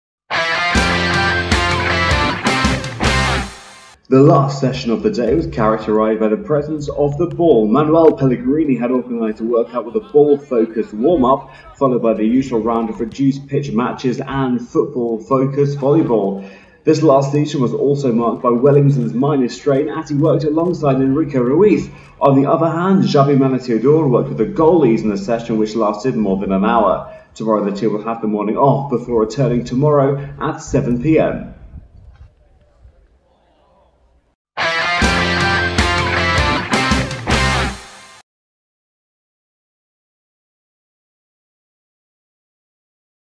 Málaga Club de Fútbol Televisión. Wednesday 13/07/11. Evening Trainning at Campoamor
13/07/2011. The last session of the day was characterised by the presence of the ball. Manuel Pellegrini had organised a workout with a ball-focused warm-up, followed by the usual round of reduced pitch matches and football-volley ball. This last session was marked by Weligton's minor strain, as he worked alongside Enrique Ruiz. Tomorrow, the team will have the morning off before returning to training at 7 tomorrow afternoon.
On the other hand, Xabi Mancisidor worked with the goalies in a session which lasted an hour.